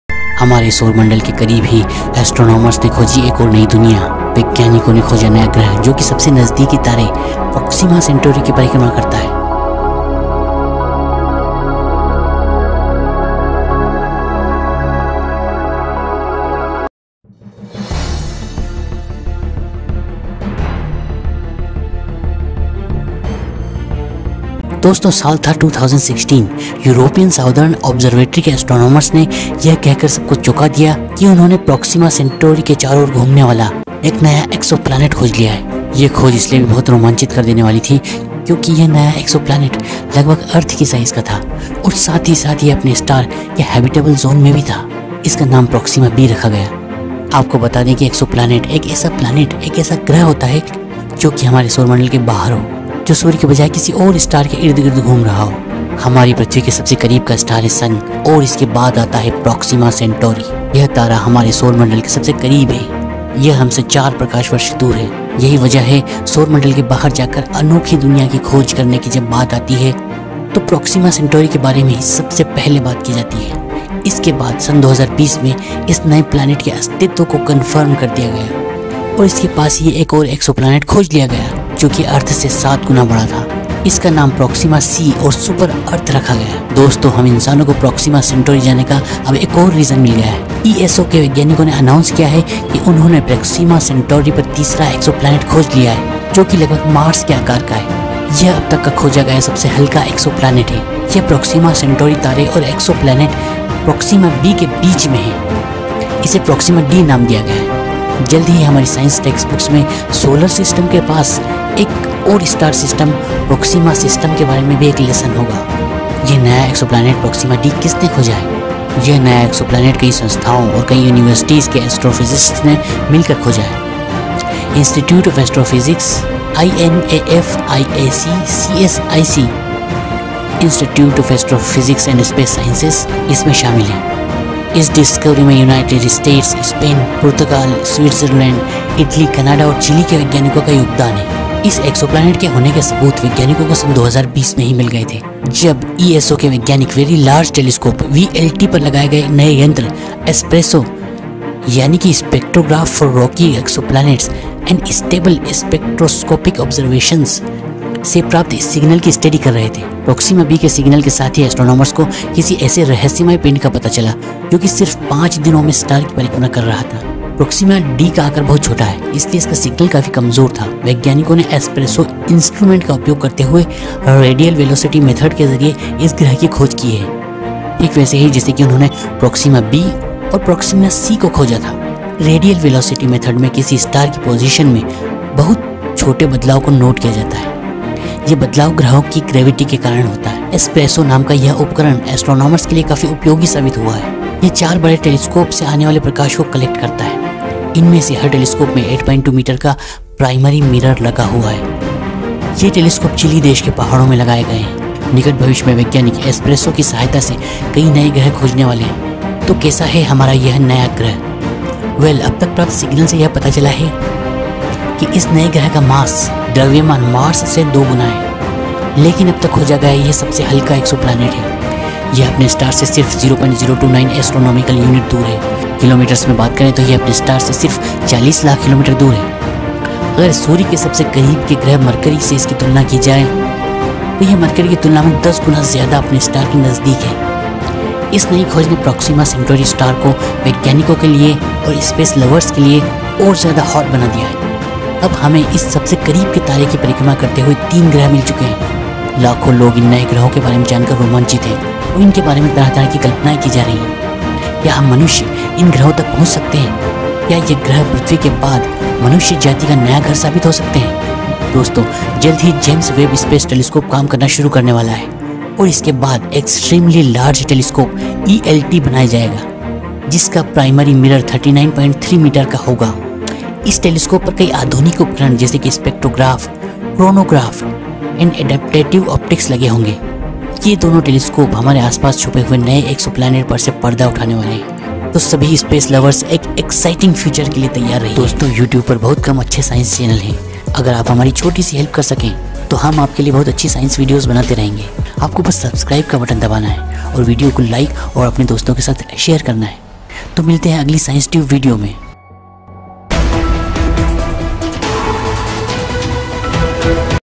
A Third Planet Found Orbiting Nearby Proxima Centauri Proxima D Hindi प्लूटो के आगे मिला नया ग्रह
हमारे सौर मंडल के करीब ही एस्ट्रोनॉमर्स ने खोजी एक और नई दुनिया
वैज्ञानिकों ने खोजा नया ग्रह जो की सबसे नज़दीक तारे प्रोक्सिमा सन्तौरी की परिक्रमा करता है ...
दोस्तों साल था २०१६, European Southern Observatory के एस्ट्रोनॉमर्स ने यह कहकर सबको चोका दिया, की उन्होंने
प्रोक्सिमा सन्तोरी के चारो और घूमने वाला, एक नया एक्सोप्लानेट खोज लिया है ... यह खोज इसलिए भी बहुत रोमाचिंत कर देने वाली थी, क्यों की यह नया एक्सो प्लेनेट, लगभग एअर्थ की साइज़ का था, और साथ ही साथ यह अपने star के हबिताब्ले जोन में भी था इसका नाम प्रोक्सिमा बी रखा गया ... आपको बता दें की एक्सो प्लेनेट एक एसा प्लेनेट एक एसा ग्रह होता है जो की हमारे सौरमंडल के बहार हो जो सूर्य के बजे किसी और star के इर्द गिर्द घूम रहा हो ...
हमारी पृथ्वी के सबसे करीब का star है सन और इसके बाद आता है प्रोक्सिमा सन्तोरी ... यह तारा हमारे सर मंडल के सबसे करीब है यह हमसे 4 प्रकाश वर्ष दूर है ... यही वजह है सौर मंडल के बहार जाकर अन्खोई दुनिया की खोज करने की जब बात आती है तो प्रोक्सिमा सन्तोरी के बारे में ही सबसे पाल्हे बात की जाती है ...
इसके बाद सन 2020 में इसके अस्तित्वे को कन्फर्म कर दिया गया  और इसके पास ही एक और एक्सो प्लेनेट खोज लिया गया जो की अर्थ से 7 गुना बड़ा था ... इसका नाम प्रोक्सिमा c और सुपर एअर्थ रखा गया ...
दोस्तों हम इंसानों को प्रोक्सिमा सन्तोरी जाने का एक अब एक और और रीज़न मिल गया है ...ESO के वैज्ञानिकों ने अन्नौंस किया है की उन्होंने प्रोक्सिमा सन्तोरी पर तीसरा एक्सो प्लेनेट खोज लिया है जो की लगभग मार्स के आकर का है, यह अब तक का खोजा गया सबसे हल्का एक्सो प्लेनेट है .. यह प्रोक्सिमा सन्तोरी तारे और एक्सो प्लेनेट  प्रोक्सिमा बी के बीच है इसे प्रोक्सिमा डी नाम दिया गया है ...जल्द ही हमारी science टेक्स्ट बुक्स में सोलर सिस्टम के पास एक और star सिस्टम.. प्रोक्सिमा सिस्टम के बारे में भी एक लेसन होगा .
यह नया एक्सो प्लेनेट प्रोक्सिमा डी किसने खोजा है? ...